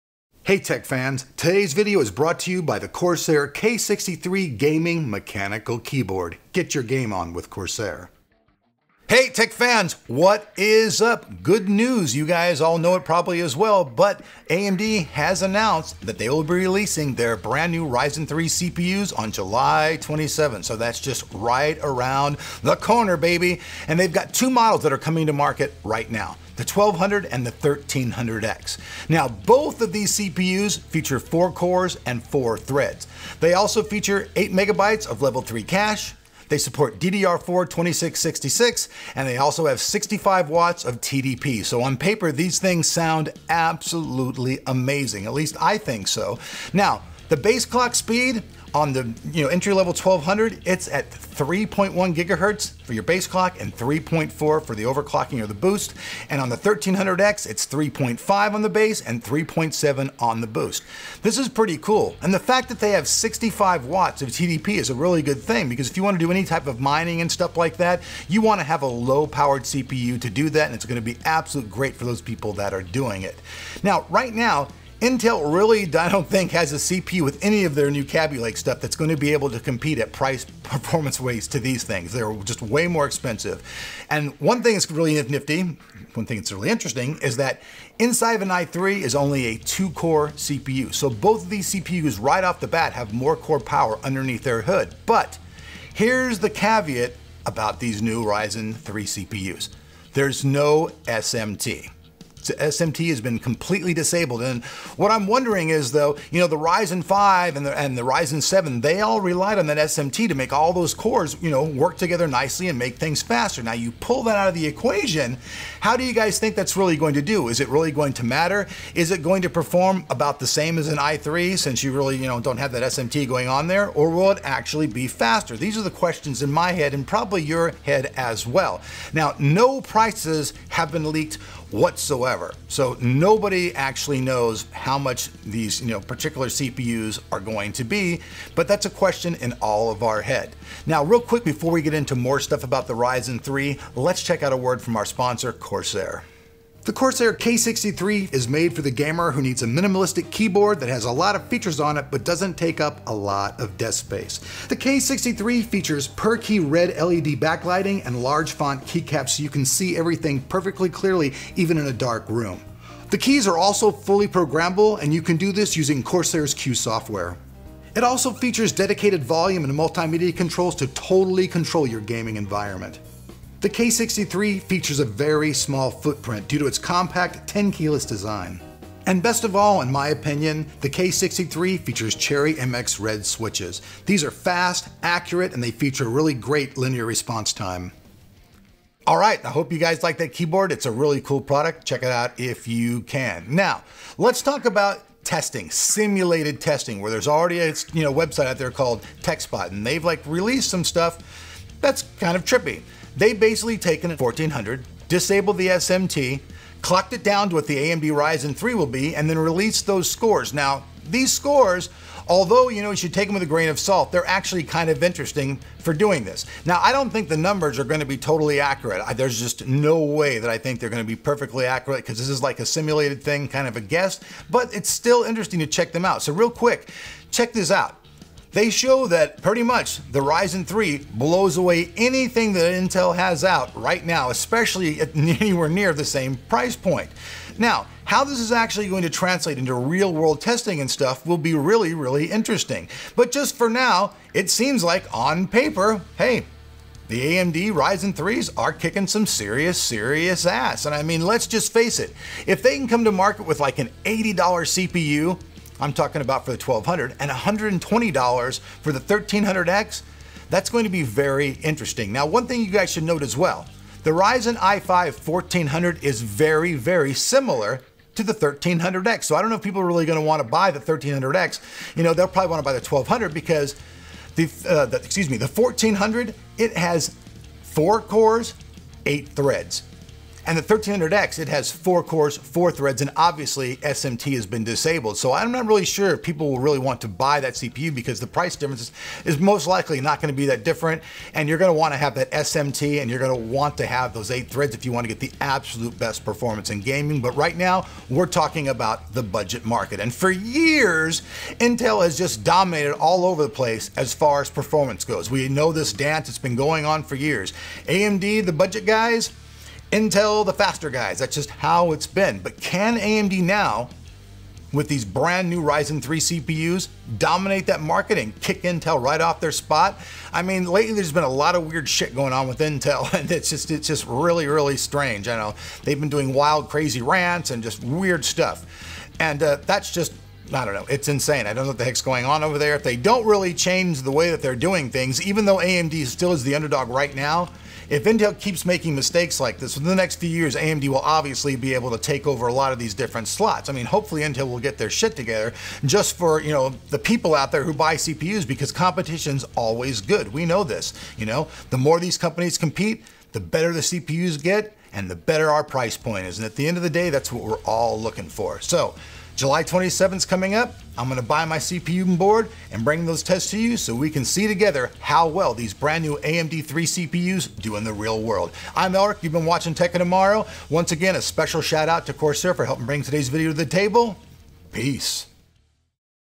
AMD Ryzen 3 1200 -1300X CPUs - What We Know So Far!
Ryzen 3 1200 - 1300X CPUs Budget Beasts Or Bust?

Corsair Gaming K63 Compact Mechanical Keyboard